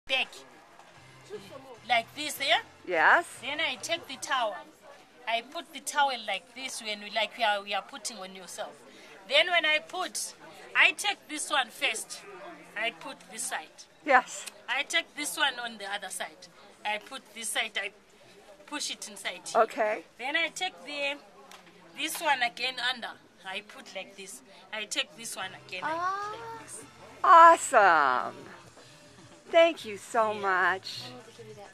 South African Lady Shares How to Securely Wrap Baby on Your Back
After a wonderful visit to Welkom in South Africa and enjoying the Free State 500 at Phakisa Circuit, the ASA group took a trip to the market place at Johannesburg. This wonderful lady was in one of the shops and was carrying her grandbaby on her back.  She was kind enough to share just how easy it can be. Now how much did I spend on that carrier for my granddaughter and this solution works just as well.